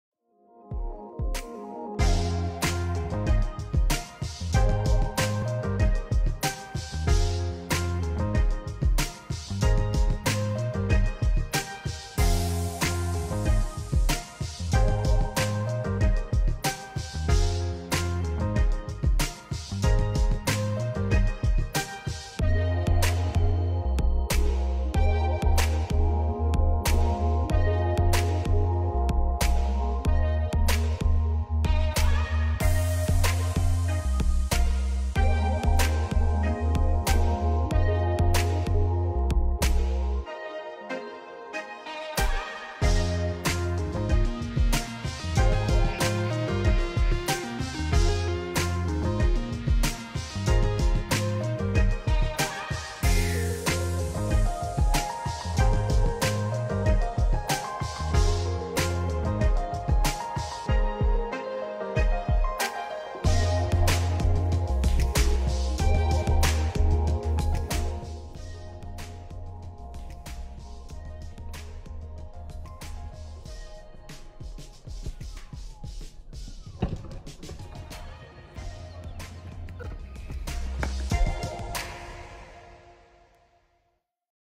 Divit Practice at Kashish Cricket Club
Welcome to my world of cricket. I am classic left handed batsman. My favourite cricket idol is Brian Charls Lara. I play for @KCC